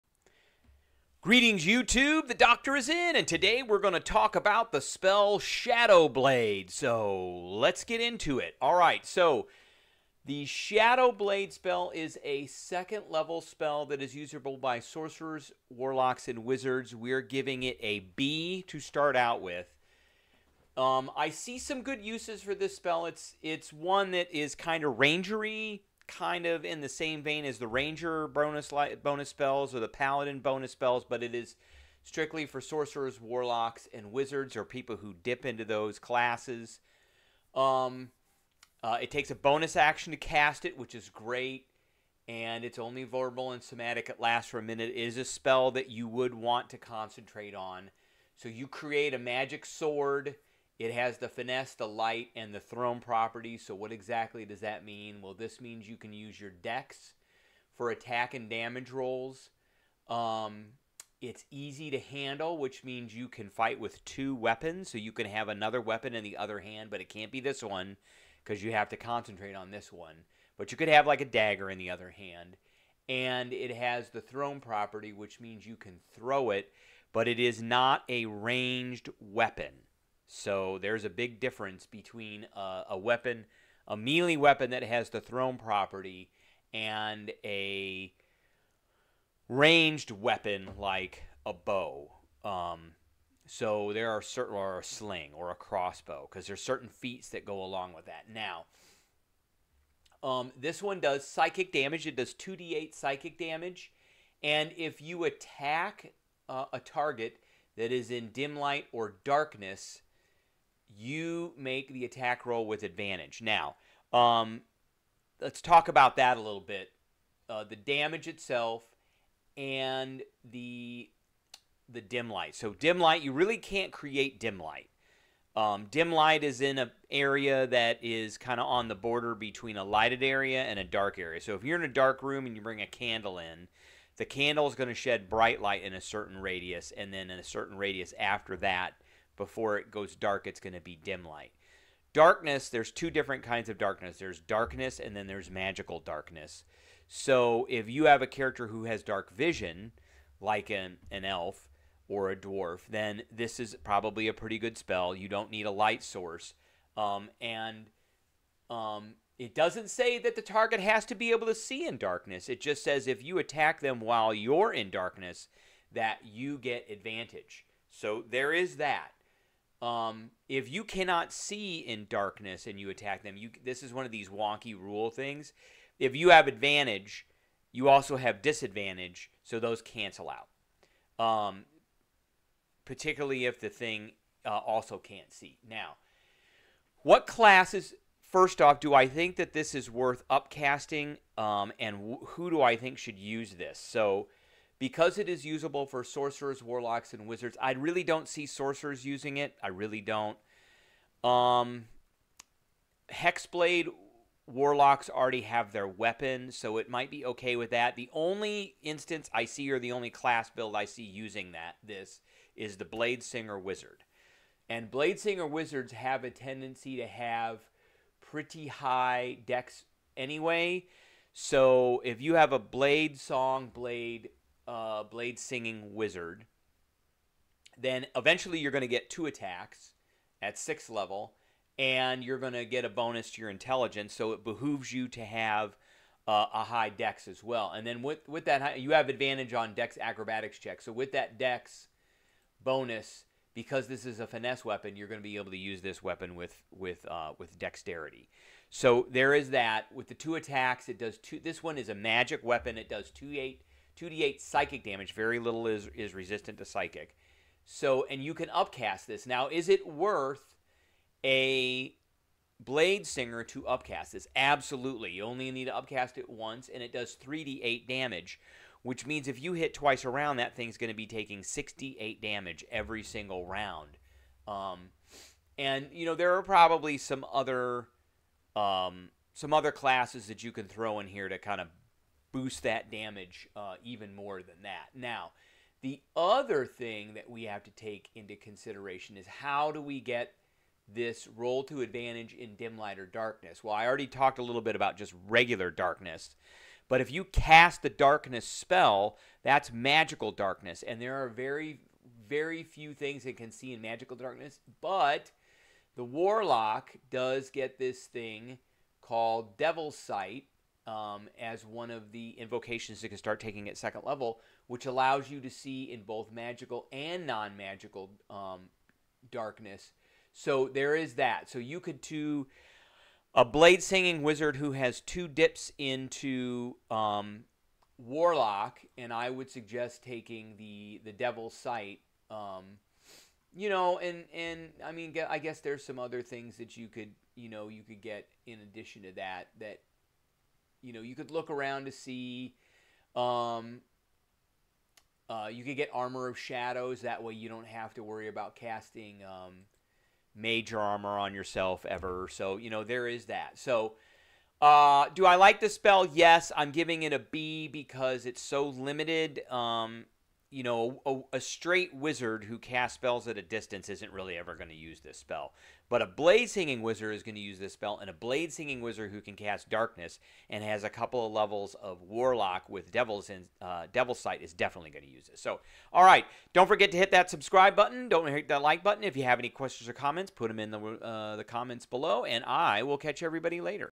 SHADOW BLADE How To Use This Spell For Dungeons and Dragons
In this video, we give our prognosis of the 2nd Level Spell Shadow Blade. Good nor Not? What is our rating? How to use? Which classes can use this and which classes should use this? Does this work with Bladesingers? Does this synergize with warlocks? How to use this spell with Devil's Sight?

for the OGL and the SRD